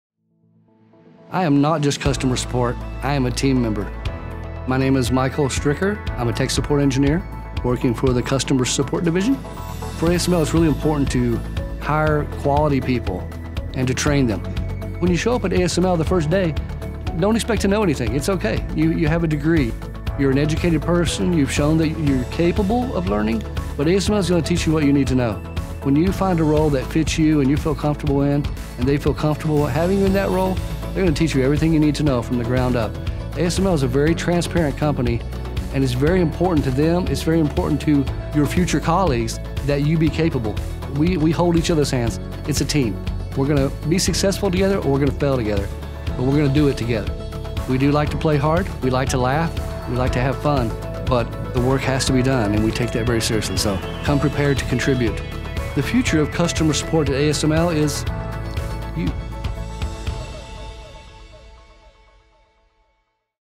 Meet Michael Stricker, Technical Support Group Lead for Customer Support
Learn more about Michael's role at ASML and how he learned each day working with other engineers. Team work is present daily whether we fail or succeed.